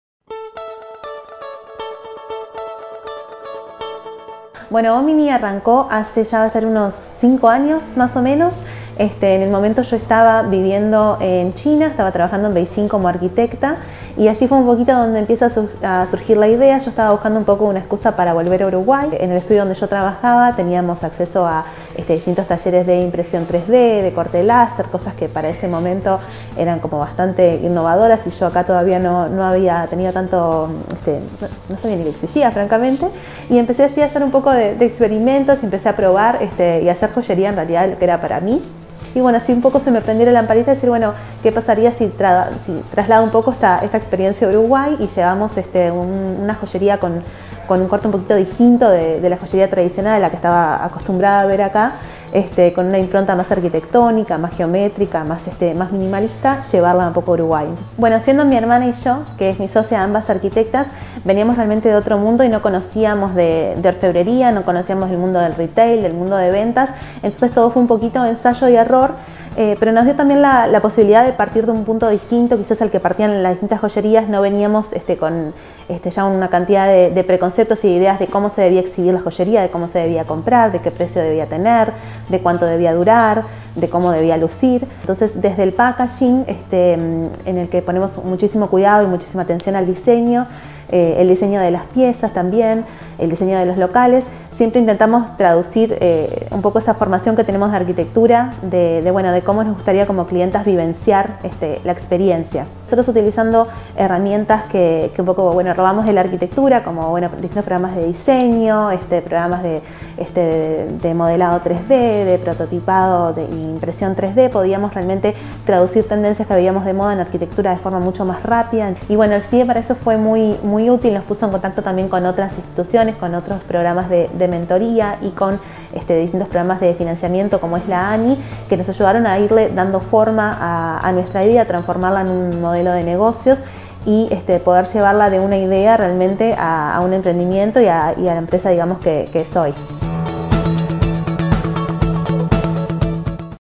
Entrevista a Deborah Kaiser. Hómini Studio
Deborah Kaiser es graduada de la carrera de Arquitectura de la Universidad ORT Uruguay. Desde hace 5 años fundó, junto a su hermana Hómini Studio, un concepto diferente en joyería en el que las arquitectas son las responsables del diseño de piezas, locales, packaging y en generar, de toda la identidad corporativa. Siempre desde una mirada compositiva arquitectónica.